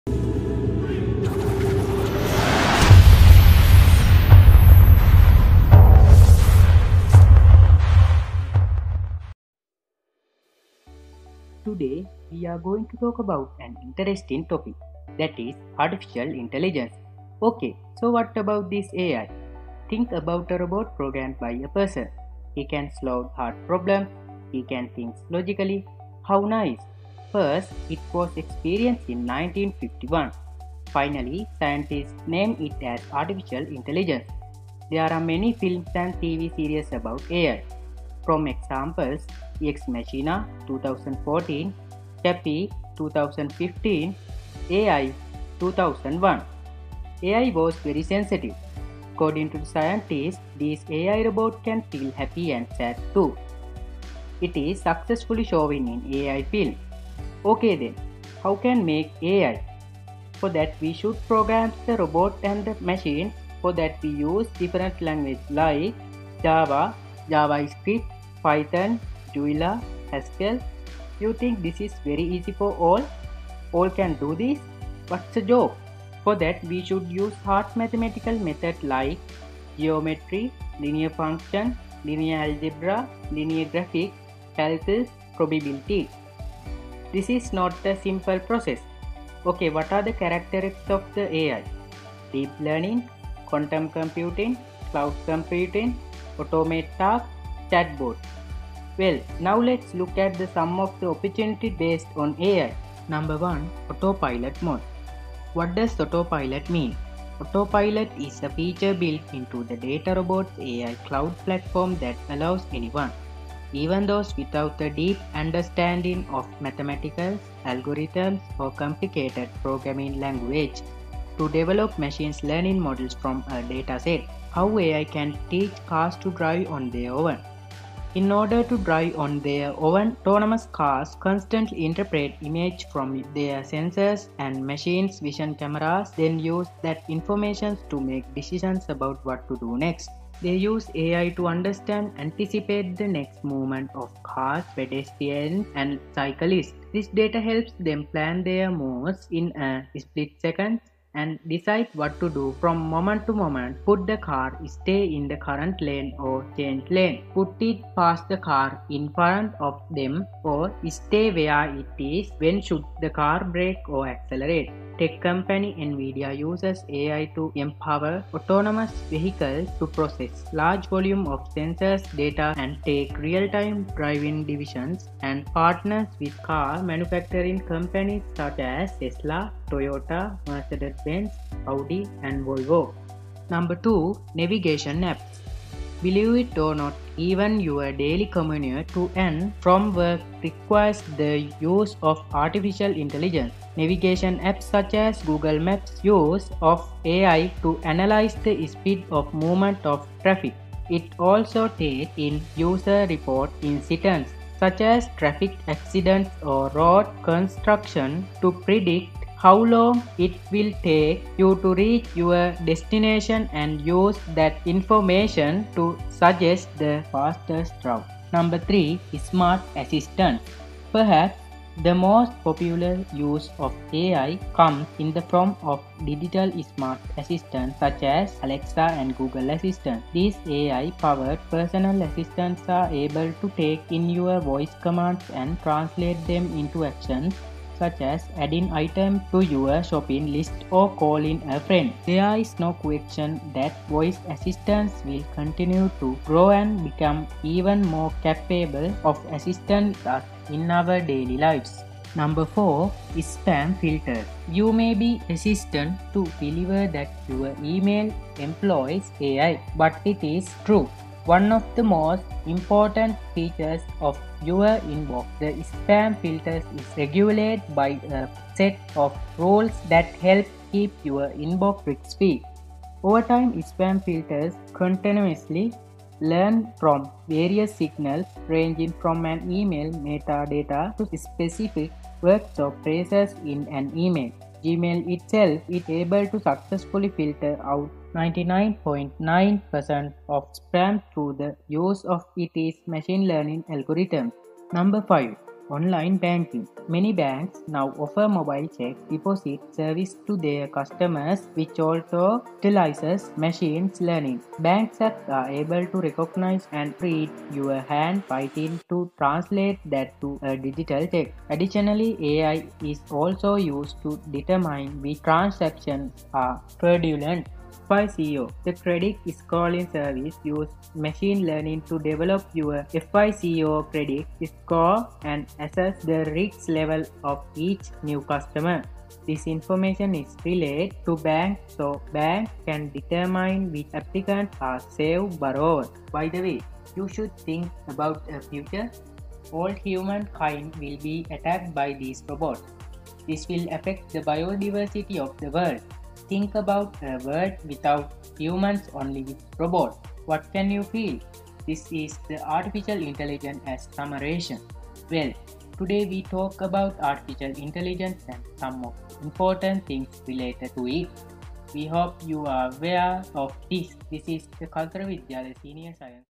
AI for Future - Kalutara Vidyalaya (Team 01)
Video 22

Visualizeara'21 - The Scientific video competition organized  by Ananda College Science Union.

Video by Kaluthara Vidyalaya - Team 01

This video is based on "Artificial Intelligence".It will be a factor that will determine the direction of the world in future.So study this video and get the attitude about it .

Members :
Yasith Kanchana
Chamadith Welarathna
K. B. Sithum Sanneth
Chathuka Dilhara

මෙහි එන සියලුම වීඩීයෝ පටවල  අයිතිය මුල් හිමිකරුවන් සතුය.
All videos here are copyrighted by the original owners.